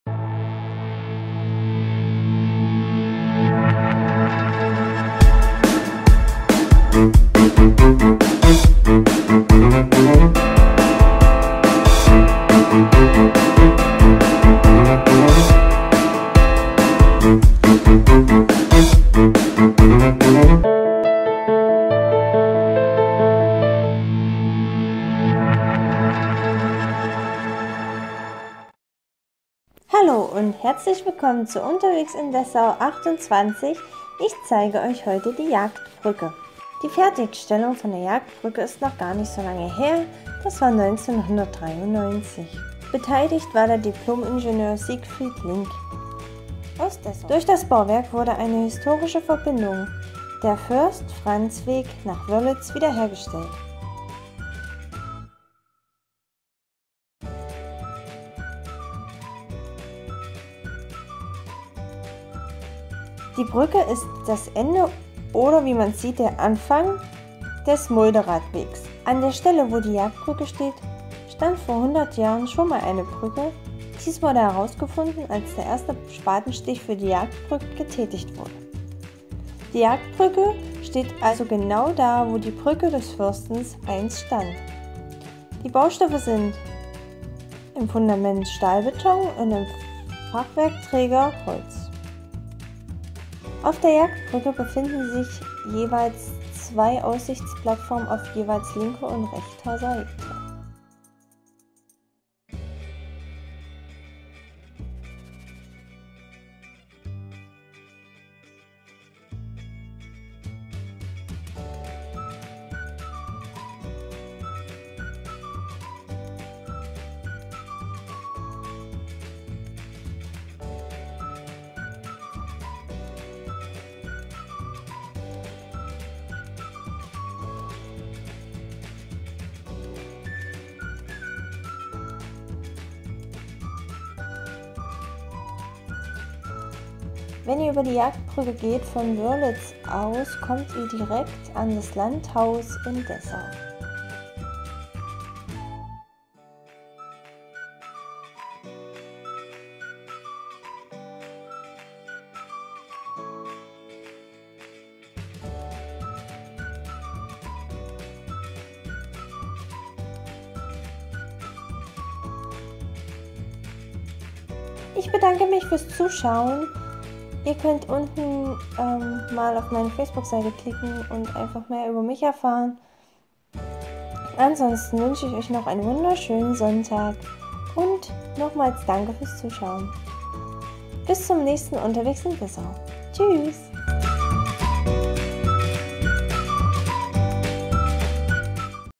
Unterwegs In Dessau 28 Jagdbrücke
Halli Hallo liebe Youtube Gemeinde!
Heute an diesem sonnigen Sonntag gibt es auch ein Video von der Sa aus Dessau!
Ich hab mal was neues ausprobiert, ich hoffe euch gefällt das.
Also ich finde es Klasse!

Findet mich auf Facebook!

Euch noch einen schönen Sonntag,
eure Sa aus Dessau